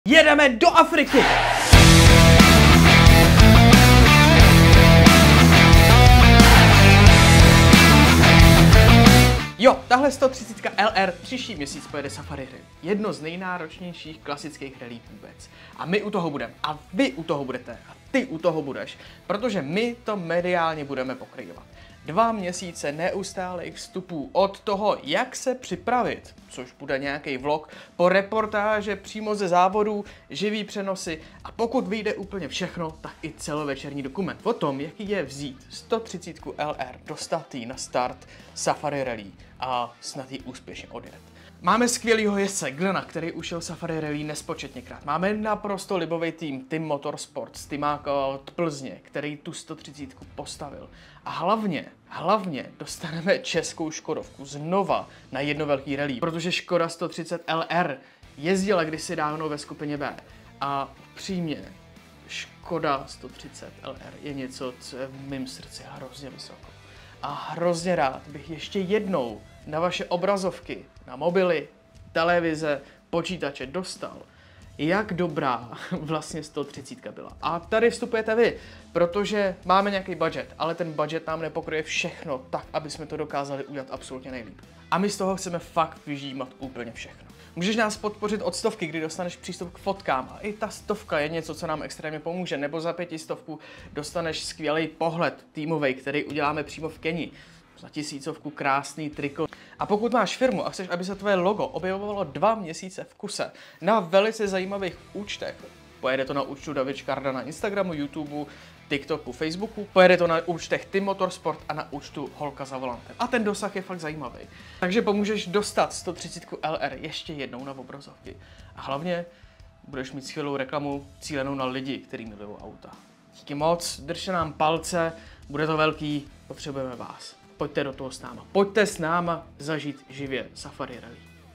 Zažij živě Safari Rally s Českou Škodou 130 LR
Vy, já, TYM-motorsport, Česká Škoda 130 LR, Safari Rally a jedna velká výzva. Díky vaší podpoře budou živé vstupy, reportáže, fotoreport a dokument o tomhle našem šíleném snu

Podpoř nás na startovači! Dostaneš speciální věci, který jinde nikdy nebudou a hlavně díky tobě vznikne něco co jinde neni. Pojď s námva vstoupit do světa Rally a zažij s náma Safari Rally se Škodou 130 LR